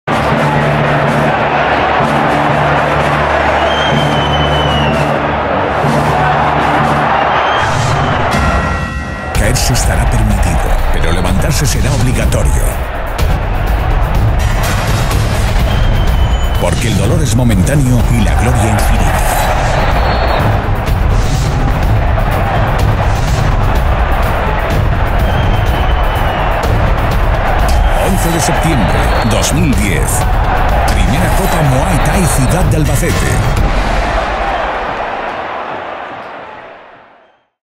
I Copa Muai Thai Ciudad De Albacete
I Copa Muai Thai Ciudad De Albacete. Sábado 11 de Septiembre de 2010. Spot realizado por Difusión De Medios. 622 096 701